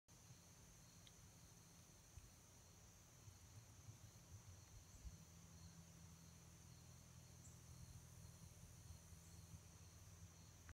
Lake Wedington, AR from Beth M.
Ring neck snake

▶︎ Beautiful park
The park is very clean and well kept. There are 18 campsites and 2 bathhouses in the campground. Nice hiking trails and very clear lake with excellent fishing. Firewood is also available for purchase. The park is very dark at night. They are full most weekends and its best to reserve online.